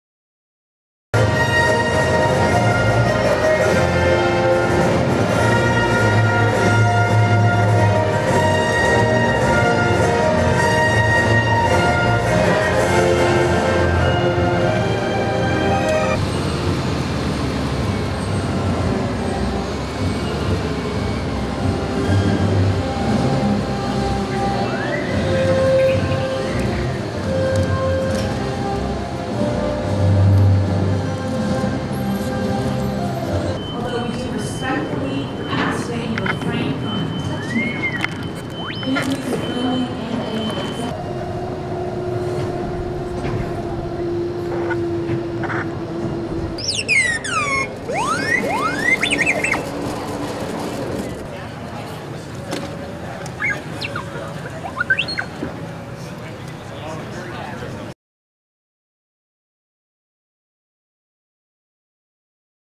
An R2-D2 Jet Plane? Check out this Star Wars Themed 787 Dreamliner
For the first time, a Star Wars character appeared on a commercial aircraft. Boeing and Japanese airline All Nippon Airways unveiled a new 787 Dreamliner with a paint job inspired by the beloved character R2-D2.